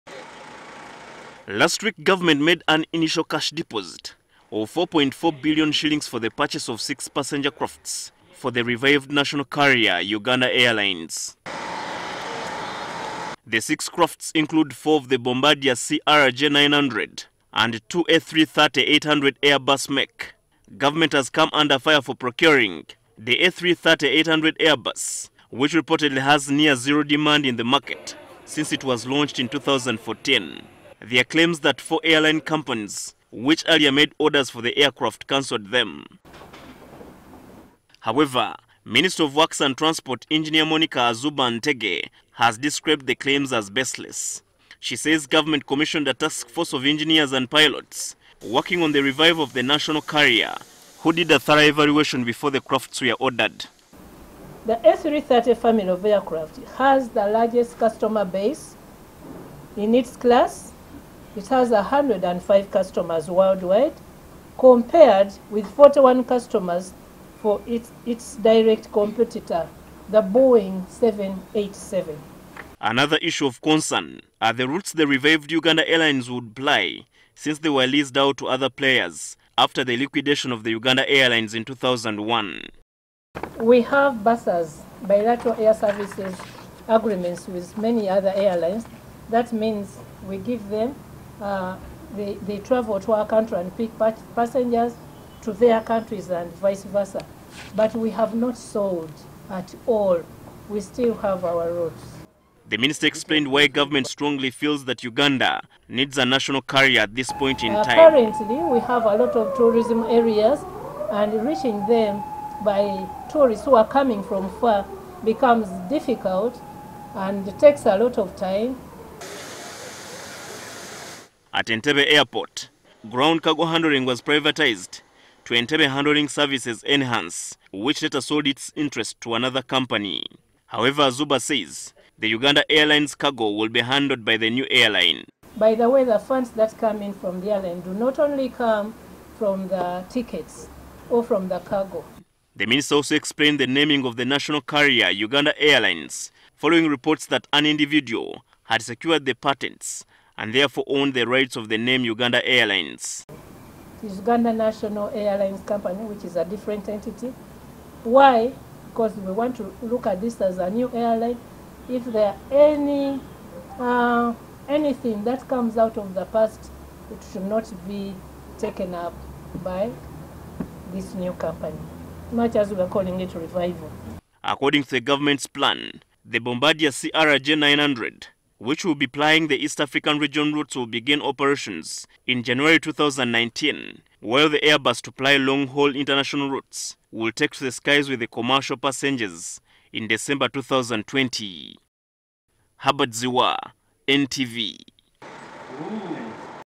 Government says the Airbus A330-800 neo was the best option for Uganda Airlines
Works and Transport Minister Monica Ntege Azubah as described as baseless claims that Uganda is procuring unwanted Airbus A330 aircraft in its bid to revive the Uganda Airlines.

Azuba says that after evaluation by an interim board the airbus A330 came out among the most preferred aircraft by the majority of airlines in its class